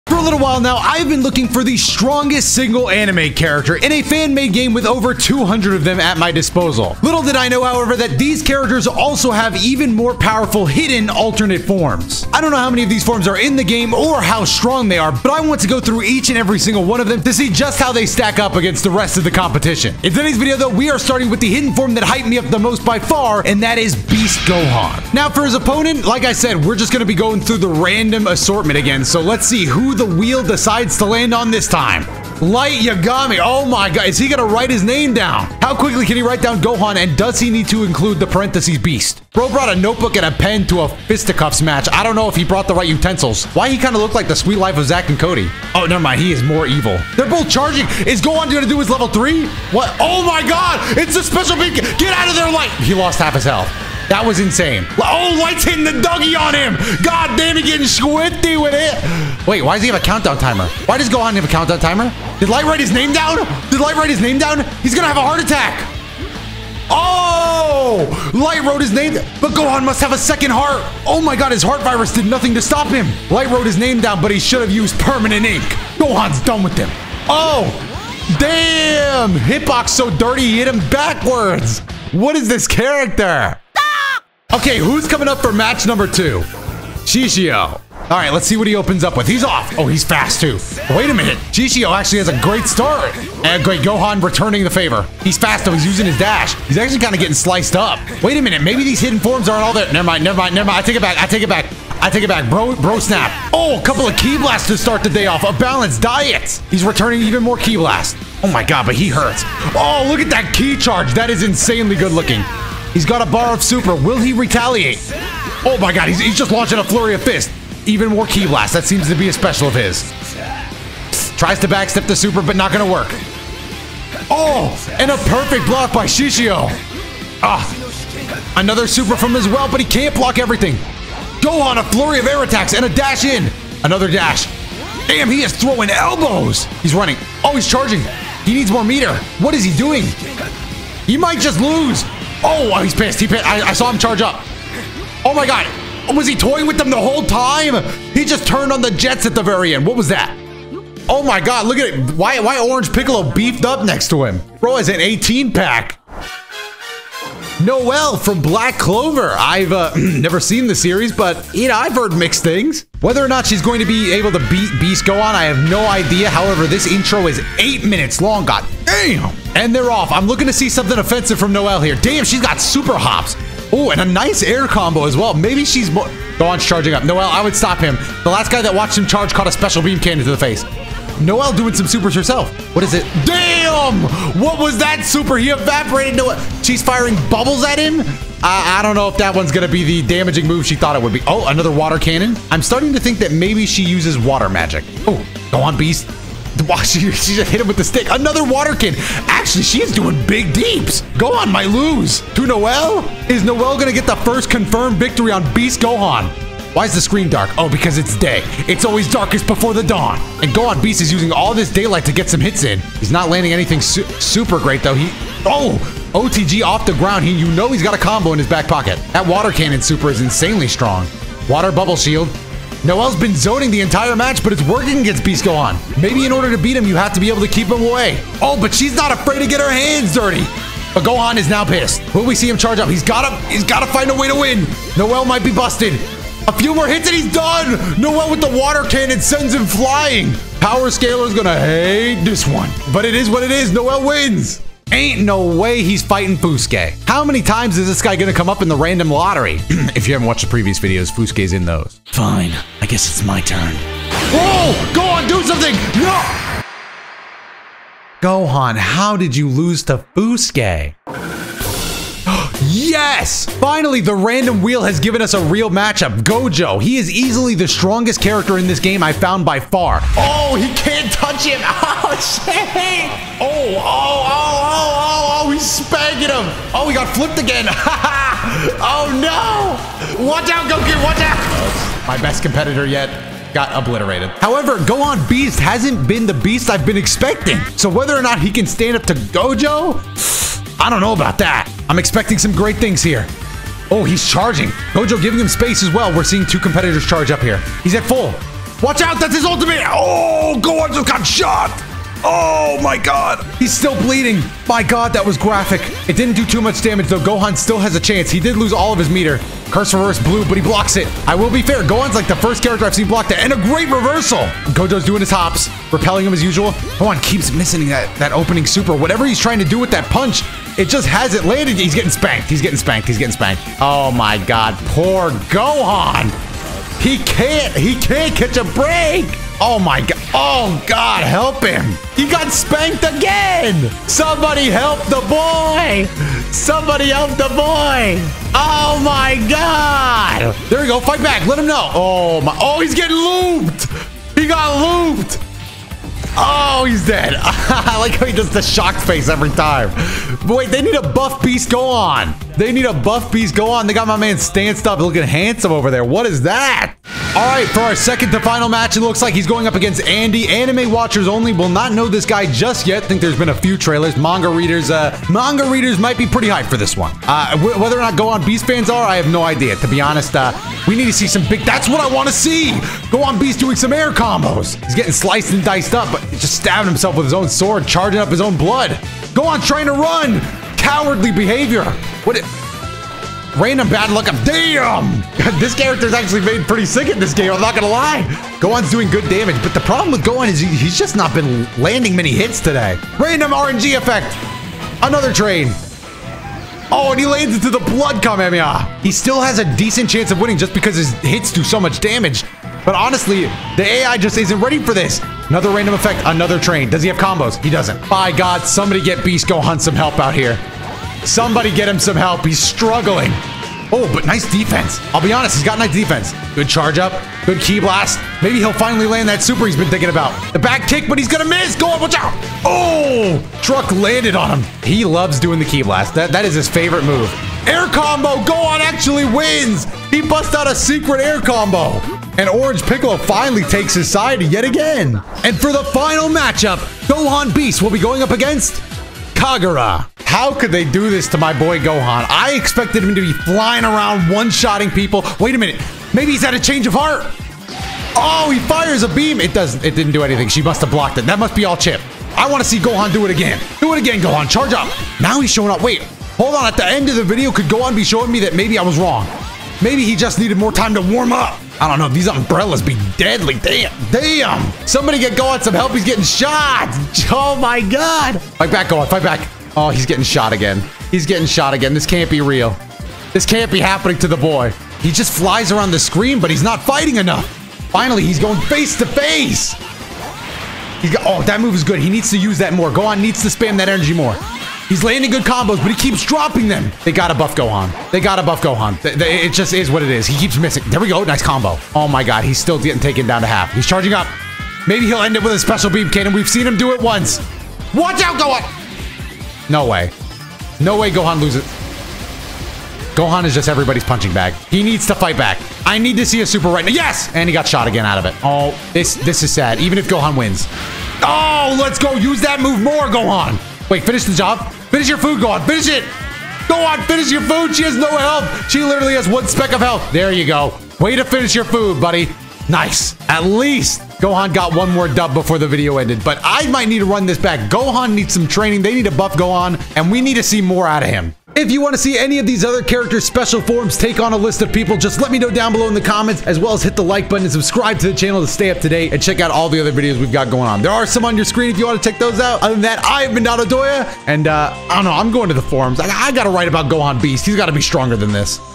They Ruined Beast Gohan
Im sad to say it but they ruined beast gohan in this game. Dont get me wrong this game is still amazing but beast gohan should be one of the best out of these 200+ characters from series like Naruto, One Piece, Bleach, One Punch Man and many more... why is he losing

Dragonball Z and Dbz game funny moments and gameplay!